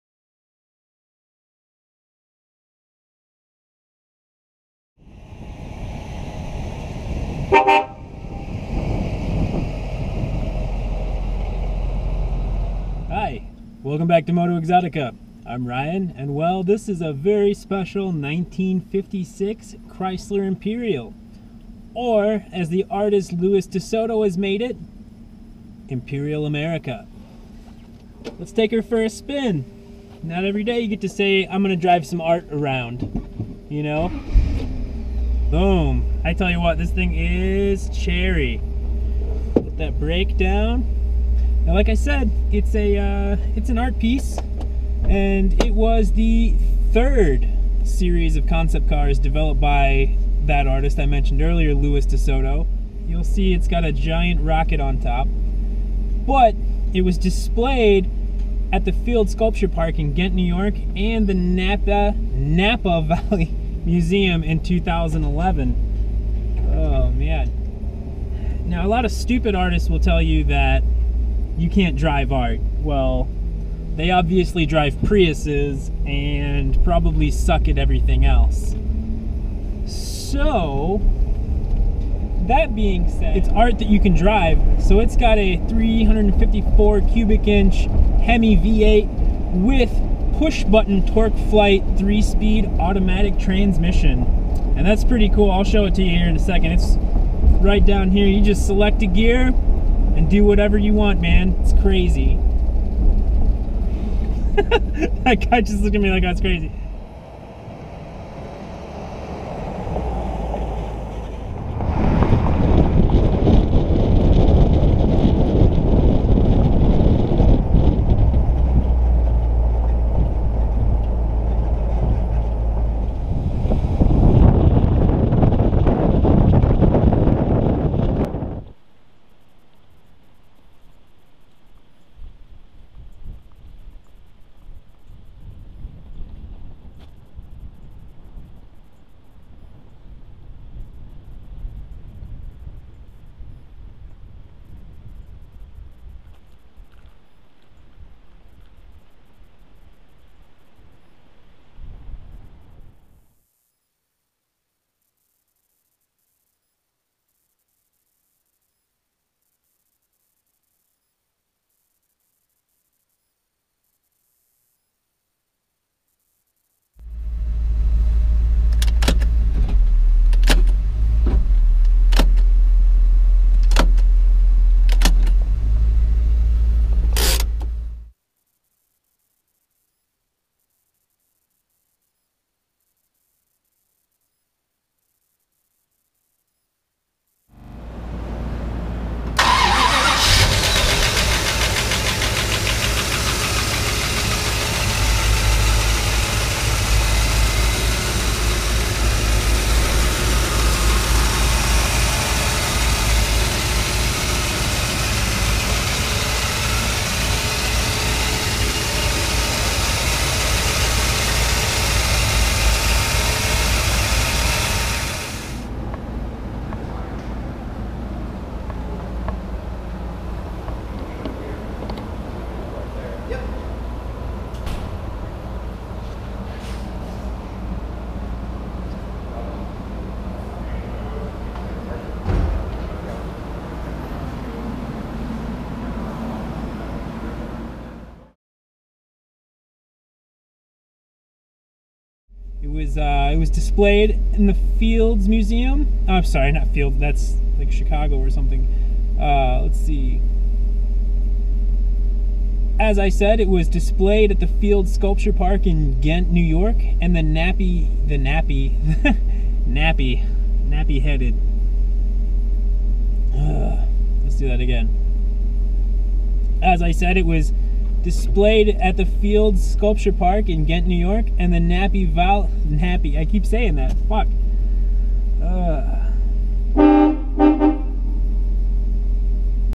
1956 Chrysler Imperial (SOLD)
“Imperial America” is the third series of concept cars developed by world-renowned artist Lewis DeSoto
    Displayed at The Fields Sculpture Park in Ghent New York and the Napa Valley Museum in 2011
    Roof mounted replica 10.5 foot scale model Chrysler CC-56 Redstone Rocket
    354ci Hemi V8 engine with a push-button TorqueFlite 3-speed automatic transmission.
    Excellent two tone West Point Gray and Cloud White exterior paint
    Stunning interior restoration from sourced NOS materials.
    California car since new, sold originally in San Luis Obispo.

MotoeXotica Classic Cars is pleased to offer this very special 1956 Chrysler Imperial from “Imperial America”, the third series of concept cars developed by world-renowned artist Lewis deSoto. The Chrysler Imperial was on display at the The Fields Sculpture Park in Ghent, New York and the Napa Valley Museum in 2011, as well as gracing the pages of The New York Times!

Lewis DeSoto has been exhibited in museums and galleries throughout the United States as well as in England, Italy, Mexico, Portugal, Spain, and Sweden. Known for public projects and sculptural installations that often incorporate sound, video and theatrical lighting, DeSoto brings to his work a strong interest in archaeology, anthropology, sociology, and world religions.

Mr. deSoto’s is customized 1956 Chrysler Imperial, illuminates the Chrysler Corporation’s little-known role in the Cold War military-industrial complex, the pinnacle of this design concept is the roof mounted 10.5-foot scale model of a CC-56 Redstone ballistic missile. Mr. deSoto is equally at home in the worlds of automobilia and conceptual art, and as a group, his concept-cars oscillate somewhere between the two.
GET OUT AND DRIVE!!!